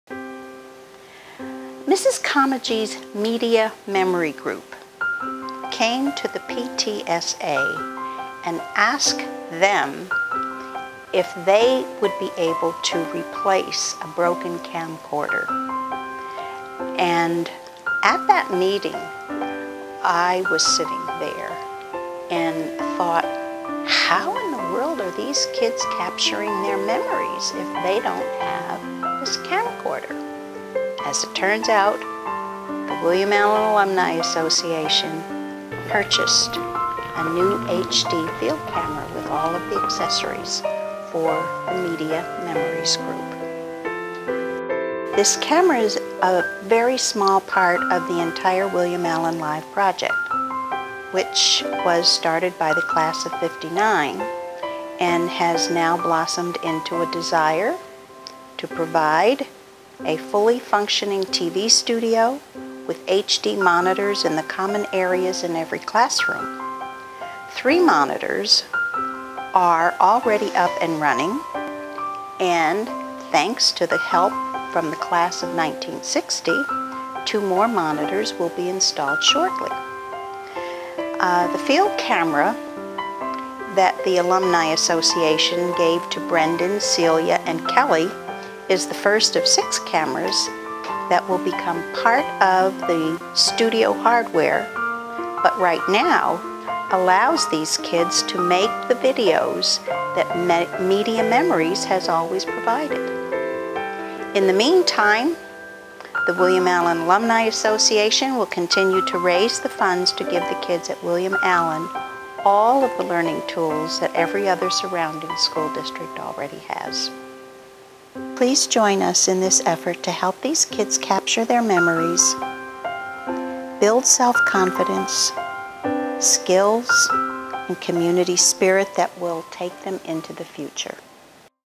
William Allen Live! Camera Presentation
Cathy Wick, Class of 1959 William Allen High School in Allentown, PA, presents students with a new camera that will help the graduating class of 2012 capture their memories.